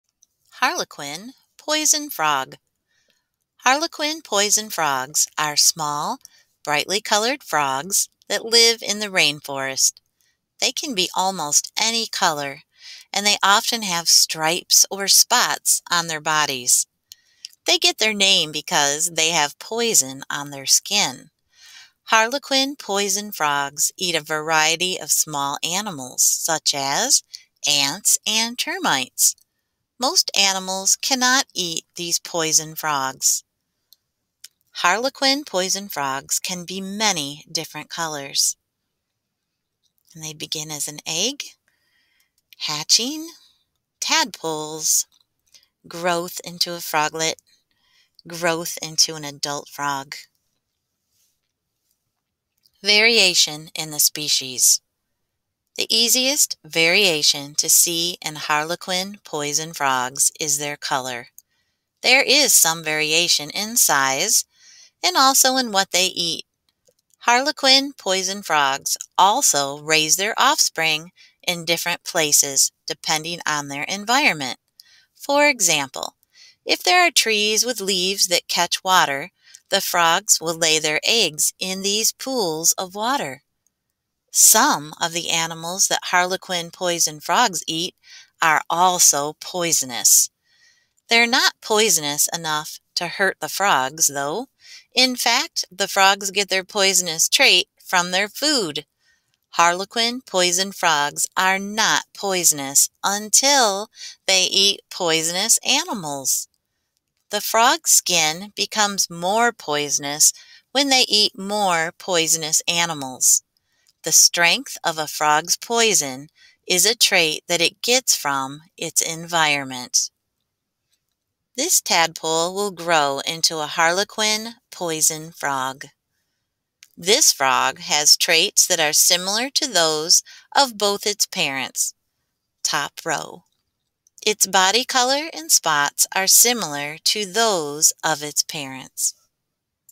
Harlequin Poison Frog: Handbook of Traits
AmplifyScience Curriculum: 3rd Grade
Unit 2: Inheritance and Traits: Variation in Wolves
Handbook of Traits
By Chloe Delafield and Jonathan Braidman
Read by Amy Richmond